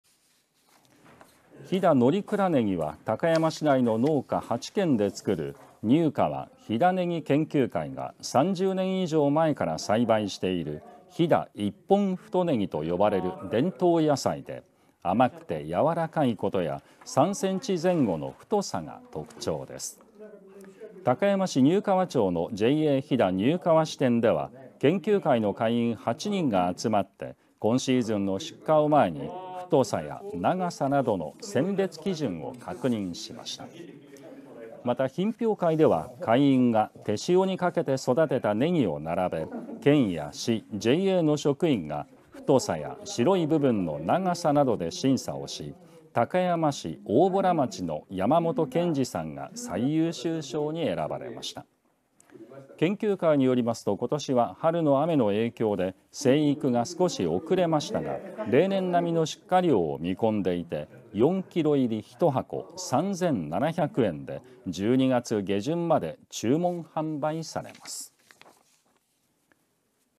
甘くて柔らかいブランドネギ「飛騨のりくらねぎ」の目ぞろえ会 岐阜県高山市丹生川町
乗鞍岳の麓の高冷地で栽培された甘くて柔らかい「飛騨のりくらねぎ」の出荷を前に、選別基準を決める目ぞろえ会と品評会が、高山市で開かれました。

ぎふチャンデジタル配信中！！